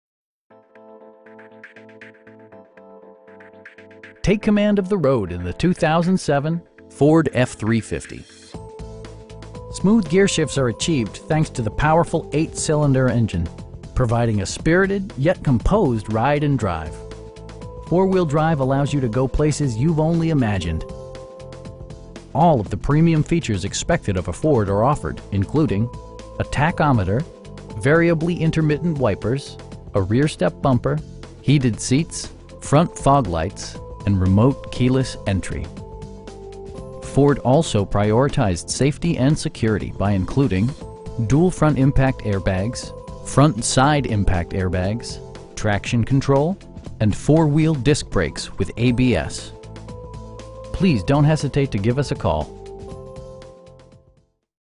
2007 Ford F-350 6.0L Diesel V8 Auto w/ Leather
2595 Barnett Highway.

Take command of the road in the 2007 Ford F-350. Smooth gearshifts are achieved thanks to the powerful 8 cylinder engine, providing a spirited, yet composed ride and drive. Four wheel drive allows you to go places you've only imagined. All of the premium features expected of a Ford are offered, including: a tachometer, variably intermittent wipers, a rear step bumper, heated seats, air conditioning, front fog lights, and remote keyless entry. Ford also prioritized safety and security by including: dual front impact airbags, front SIDE impact airbags, traction control, and 4 wheel disc brakes with ABS. We pride ourselves in the quality that we offer on all of our vehicles. Please don't hesitate to give us a call.